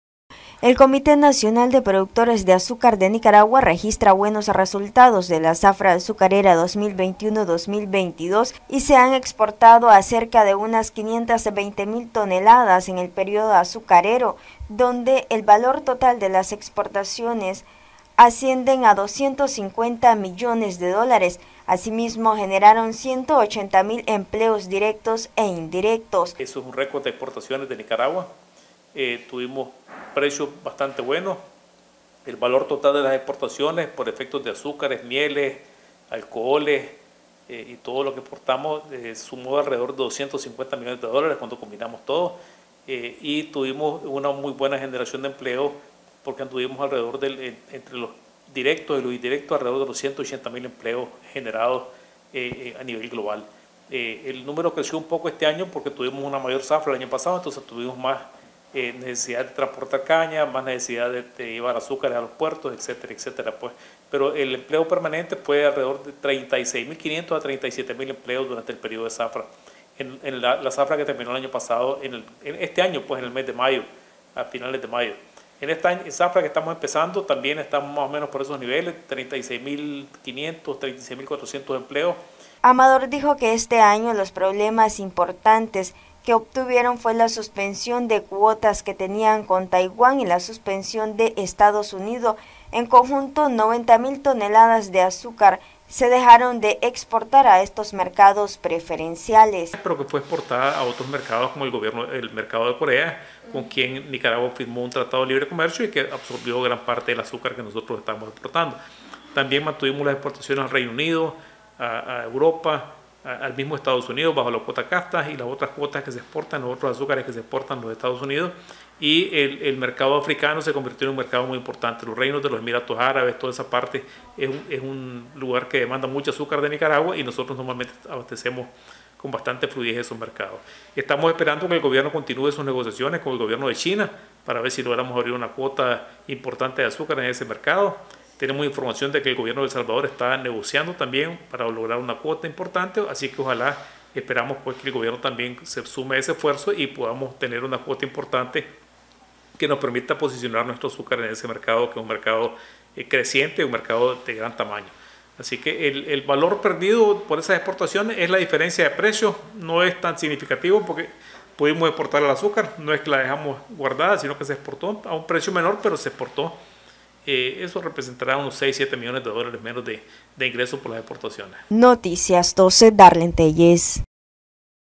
Unos 250 millones de dólares fueron las exportaciones del periodo azucarero
Mario Amador Comité Nacional de Productores de Azúcar de Nicaragua, dijo que las exportaciones de este año ascienden a 250 millones de dólares